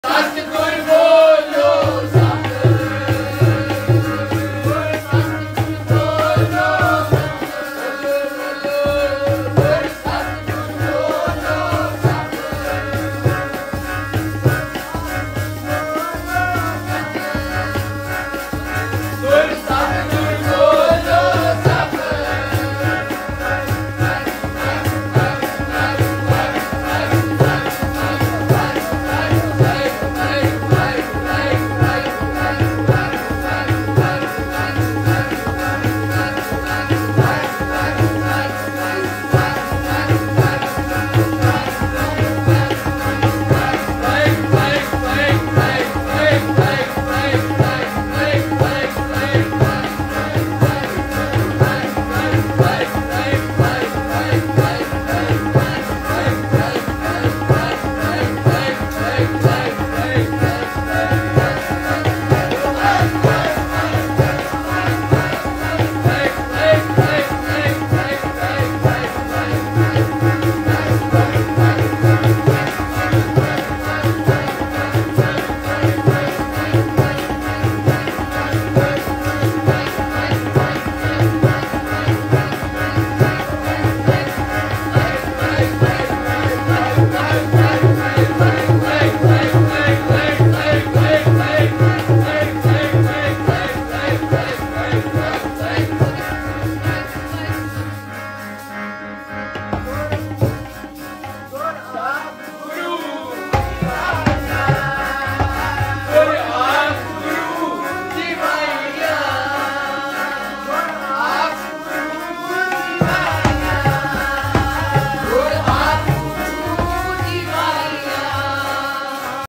Bhai Harpreet Singh Ji - Akhand Jaap Rainsabai 21/10/23
Sikhs in UAE 🇦🇪 regular weekly samagams.

Note: These are Sangat programs, everybody is invited to take blessings of Sahib Shri Guru Granth Sahib Ji Maharaj